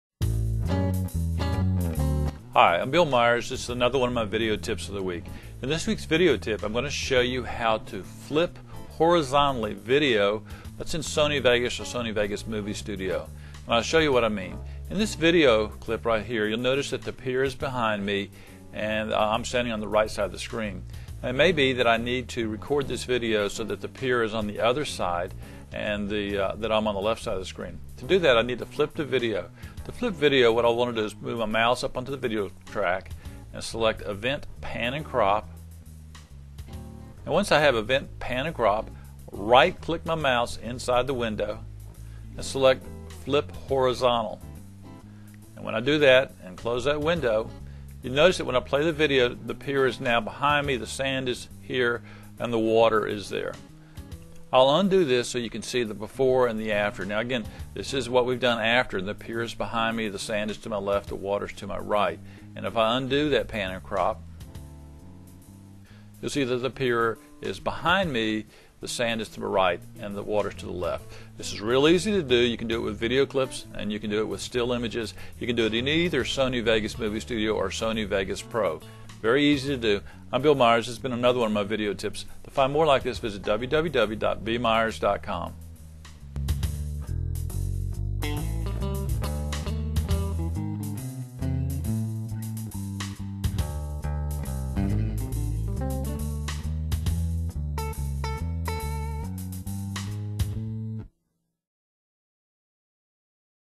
Horizontally flip video in Sony Vegas Movie Studio
Here's how to change the perspective or horizontal direction of a video clip in sony movie studio or vegas